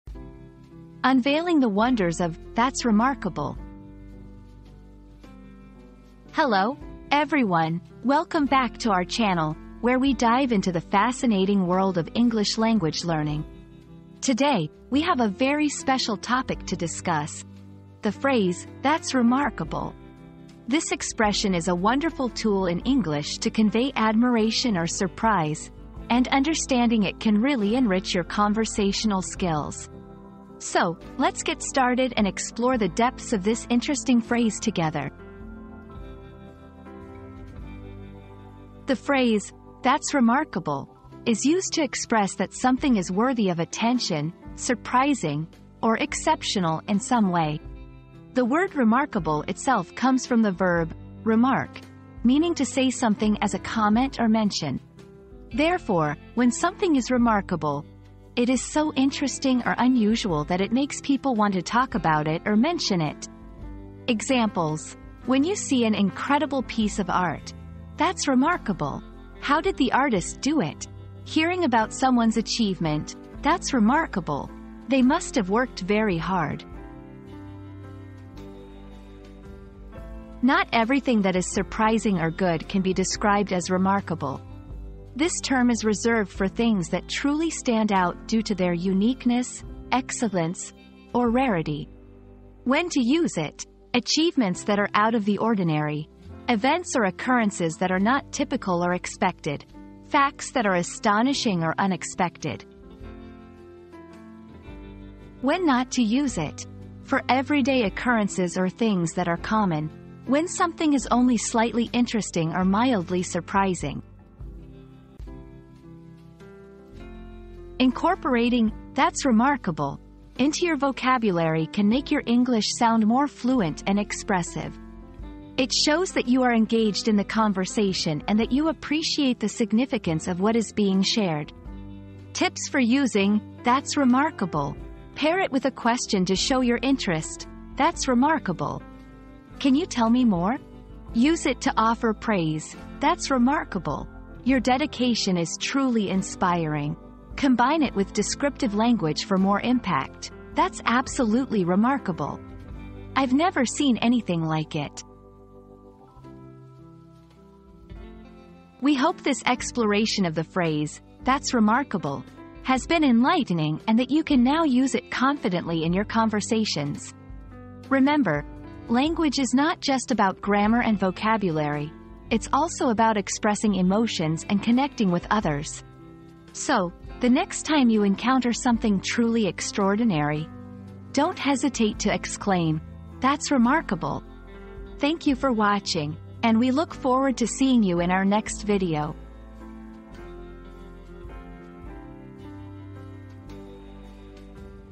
Unveiling the Wonders of "That's Remarkable!"
00:00 • Introduction - Unveiling the Wonders of "That's Remarkable!"
00:38 • Understanding "That's Remarkable!"
01:19 • The Nuances of Remarkability
01:57 • Enhancing Your Conversational Skills